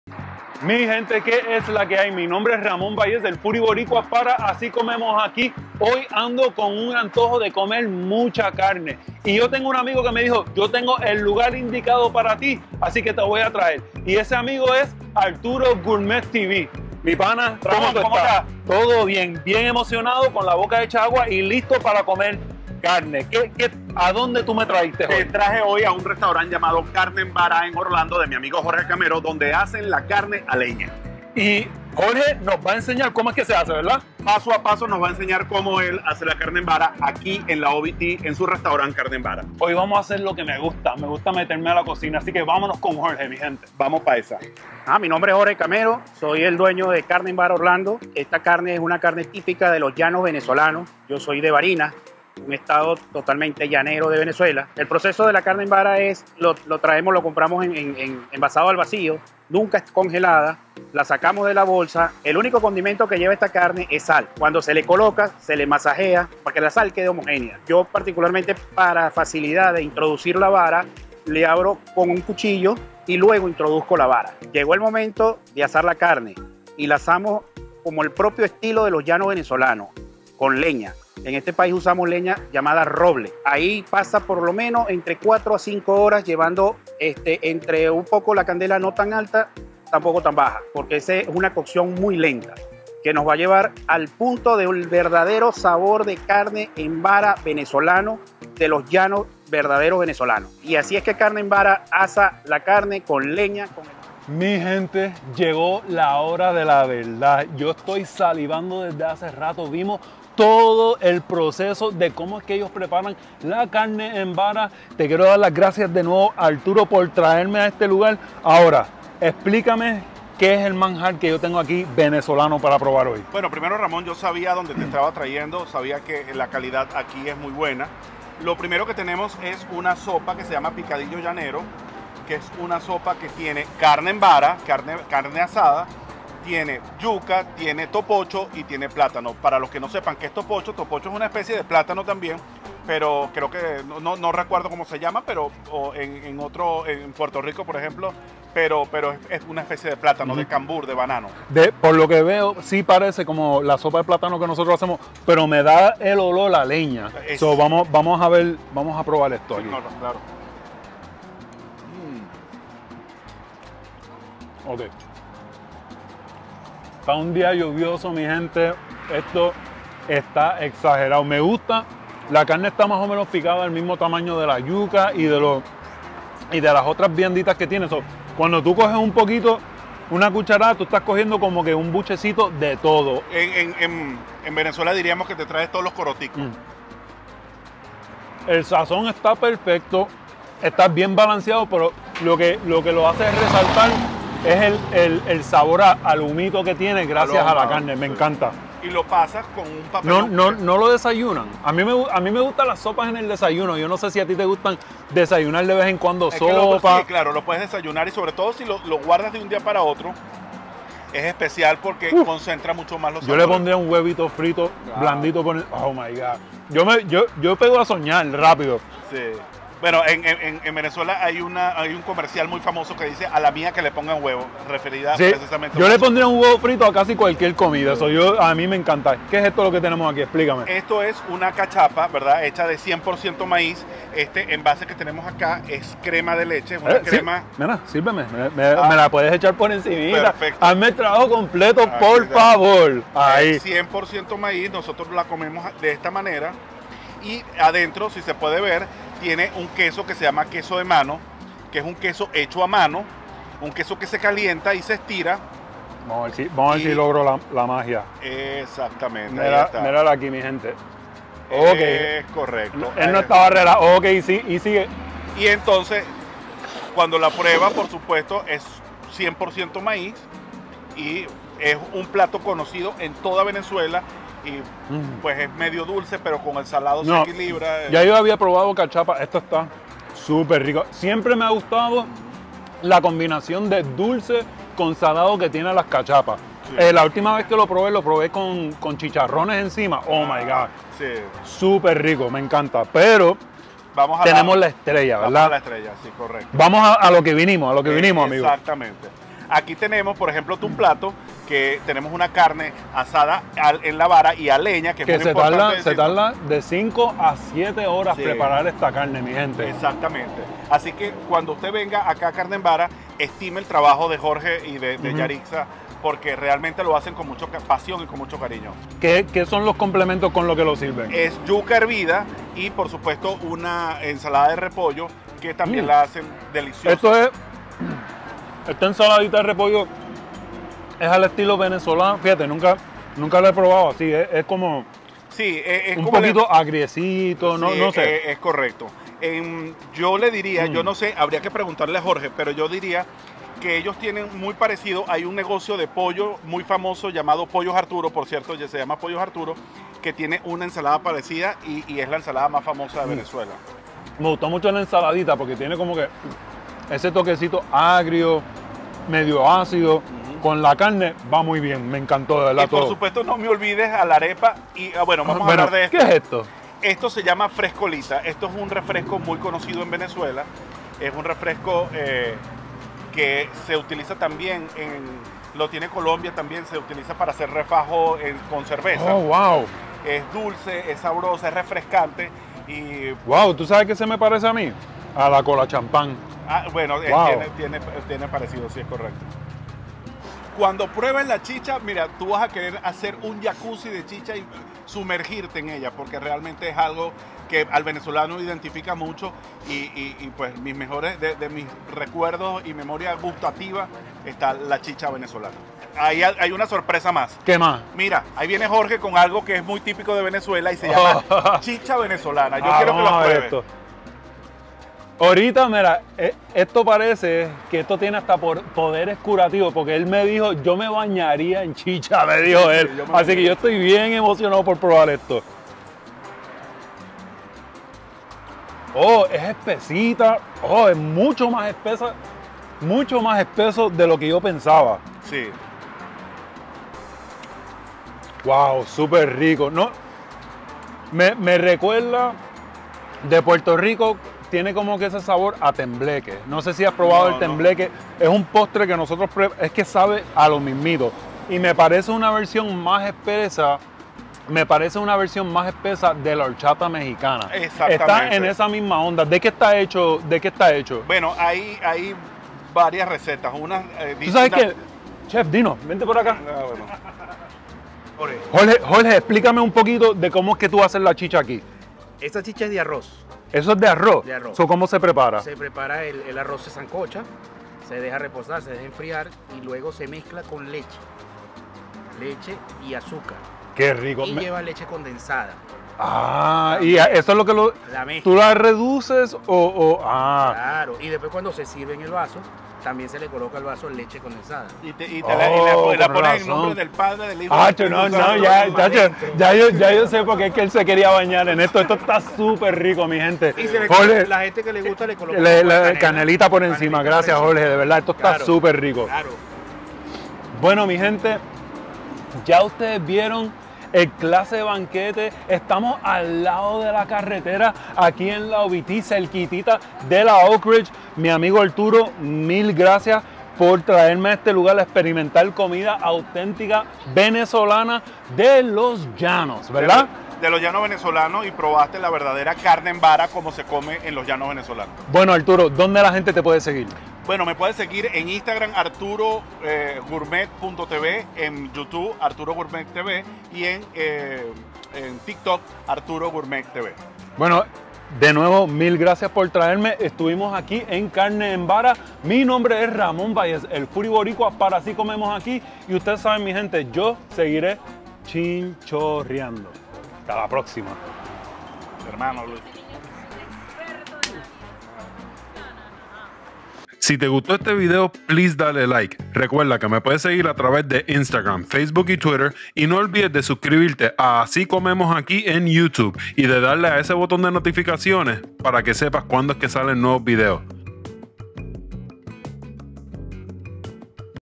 Probando Carne En Vara Orlando
Estoy en Carne en Vara Orlandofl para probar el sabor autentico de loa llanos de Venezuela 🇻🇪 junto a mi amigo Arturo Bello.